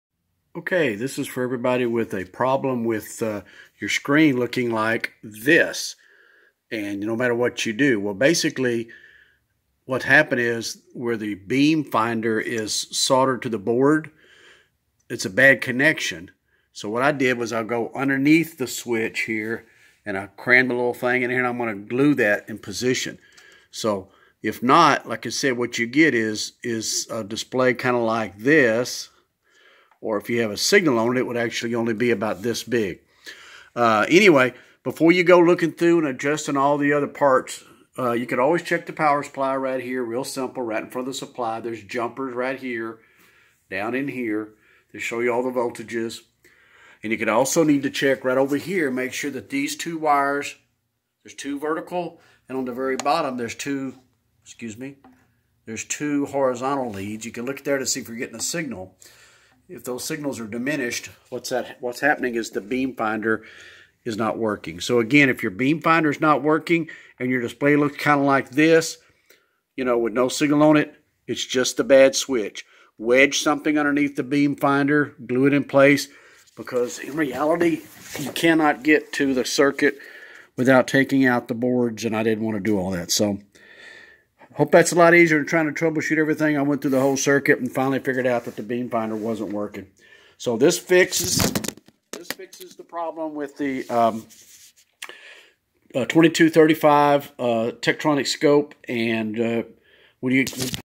Tek tektronix 2235 horizontal display fix
Beam finder circuit causing horizontal to not fill screen fully.